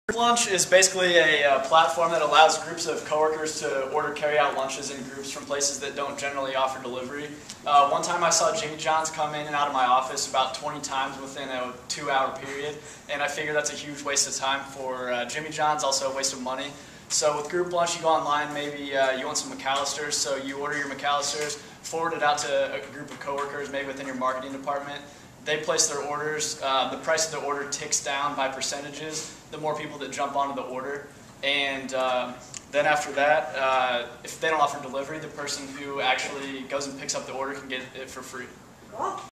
Eric (2)
A service that allows groups of co-workers to order carry-out lunch from places that don't typically offer delivery.